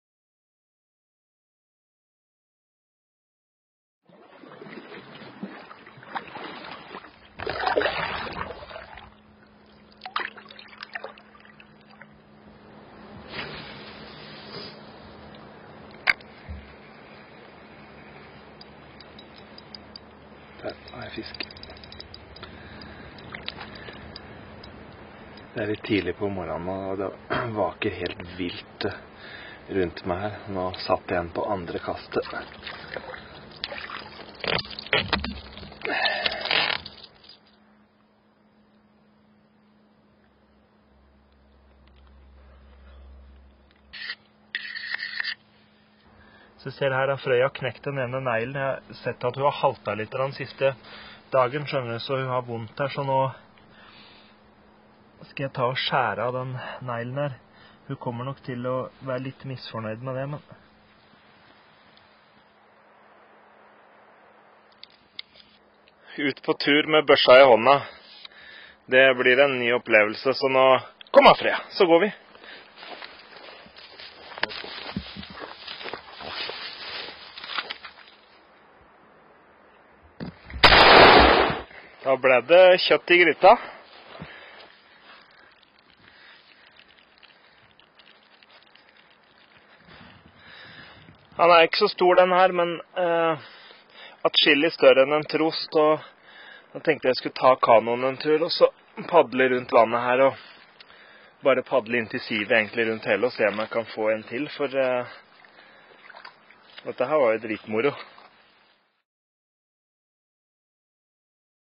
En vill mann nr 1. med Kristoffer Clausen
Kristoffer Clausen skal forsøke å overleve et helt år i Norsk natur på kun det han skyter, fisker eller finner av andre spiselige ting i naturen. Her er første smakebit fra prosjektet.